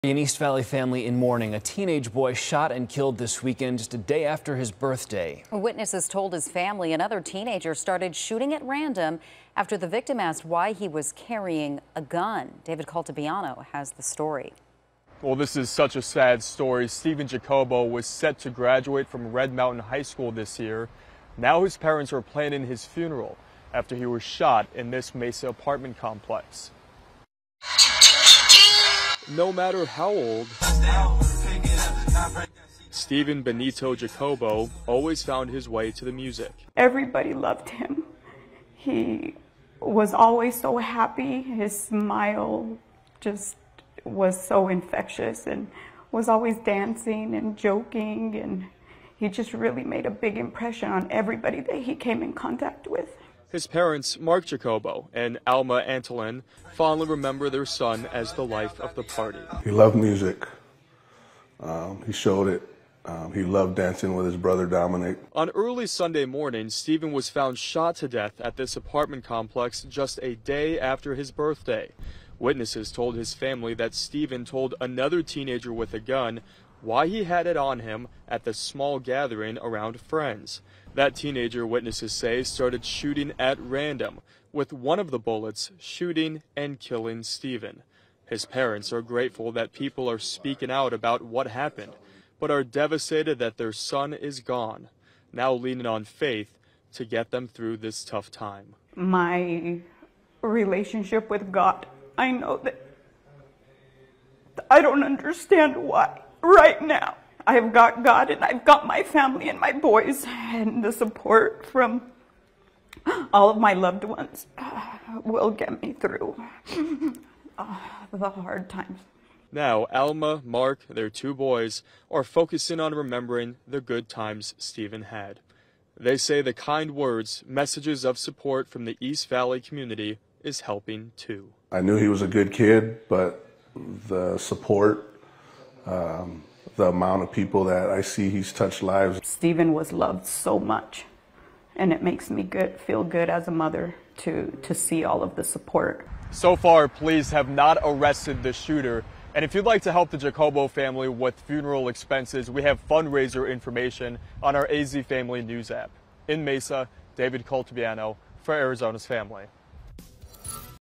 East Valley family in mourning after teen shot, killed just one day after his birthday
His parents, Mark Jacobo and Alma Antolin remember him as being the bright light in the room and the life of the party.